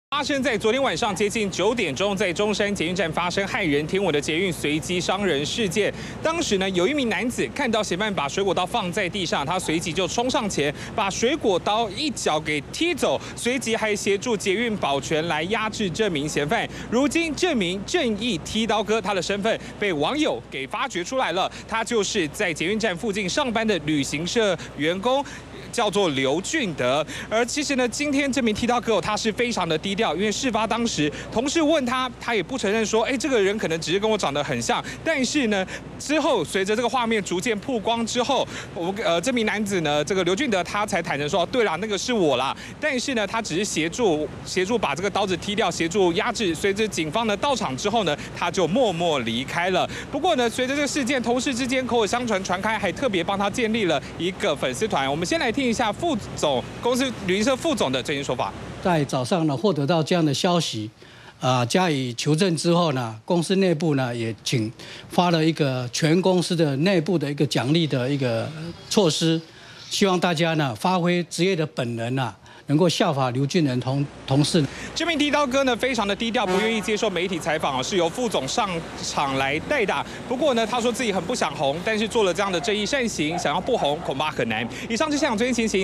20150721中天新聞 熱血踢刀哥身分曝光 公司記功發獎金
20號晚間發生在中山捷運站隨機砍殺事件，當時有名男子看到嫌犯放下水果刀，立即上前把刀踢走，還協助壓制，事後被人發現原來他是附近旅行社的職員，詳細情形連線記者李家名。